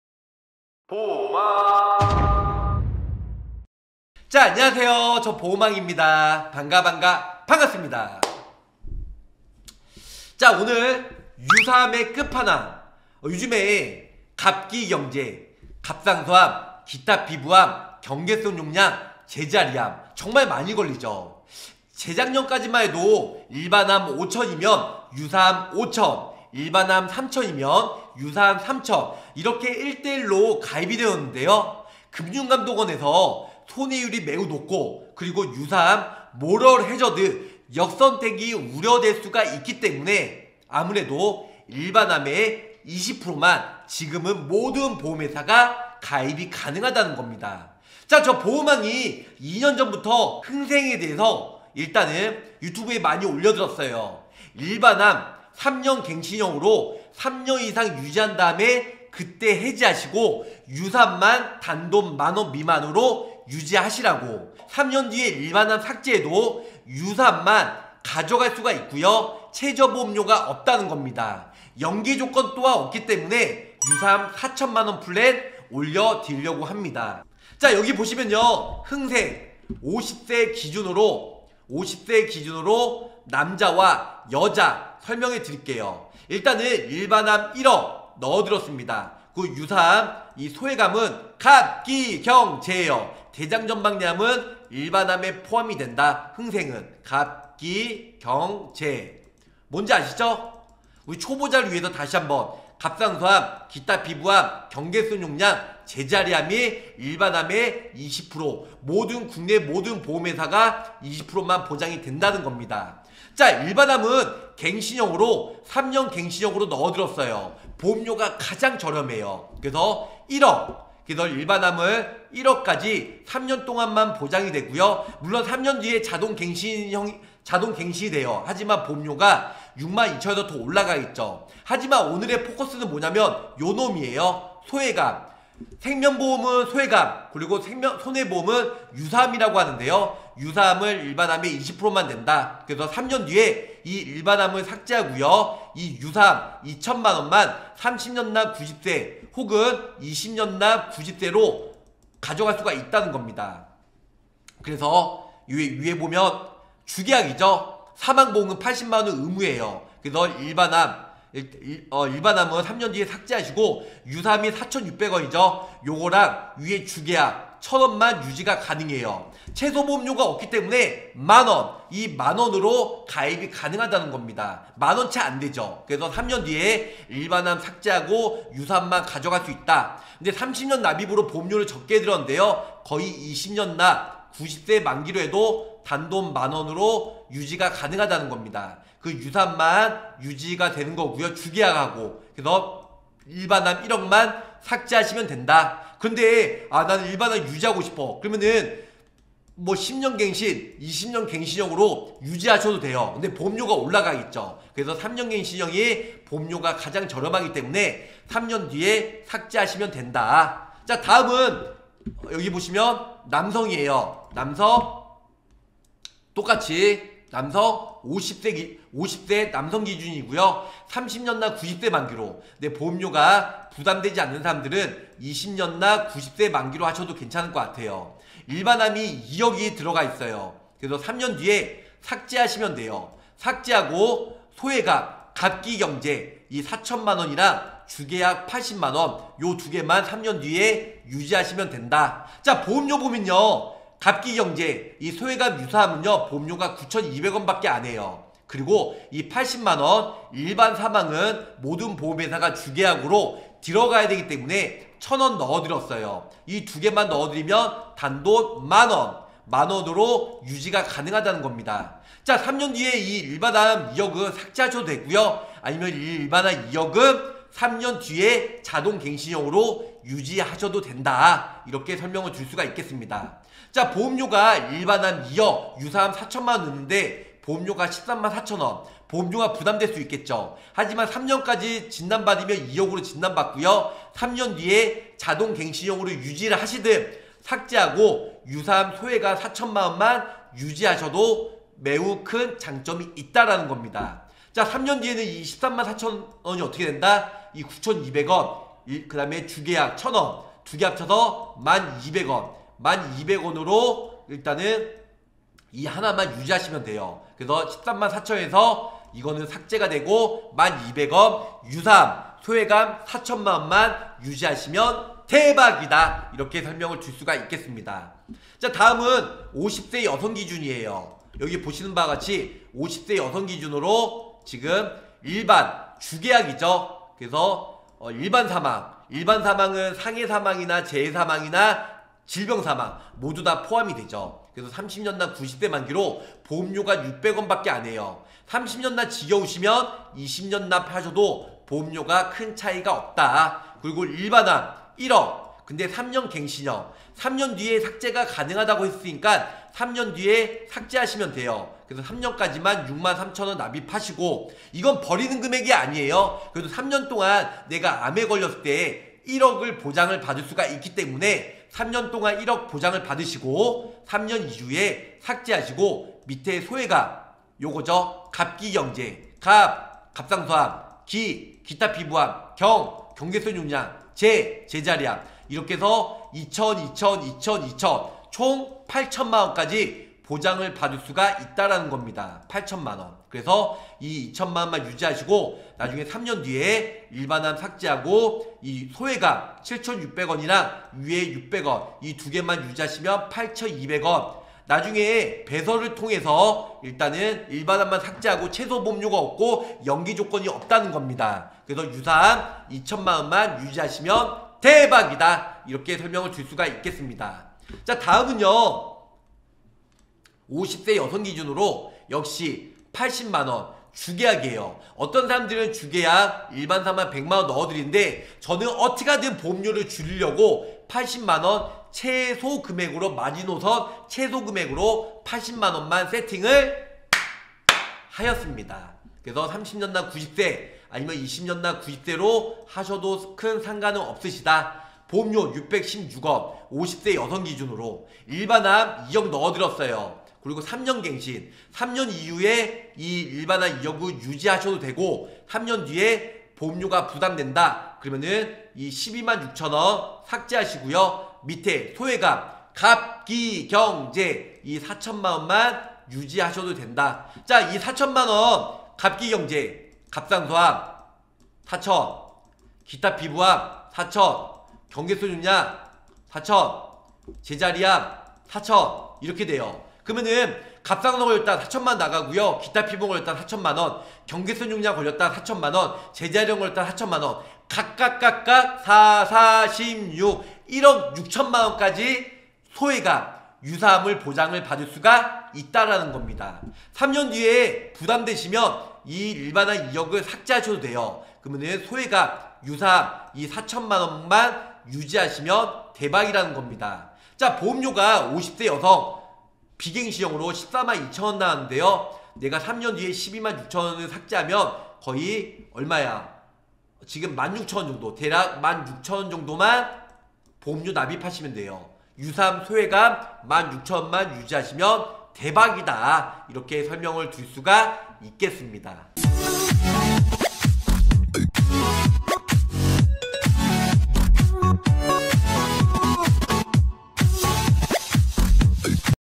가장 걸리기쉬운 유사암 총1억6천만원플랜!! (갑상선암4천 경계성종양4천 기타피부암4천 제자리암4천 각각지급~)
안녕하세요~~

가장 걸리기쉬운 유사암 총 1억6천만원플랜 올려드립니다
비교를 통해 준비하시기 바라며

감사합니다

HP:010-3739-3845
카톡친구추가:r2an
카톡플러스친구:보험왕검색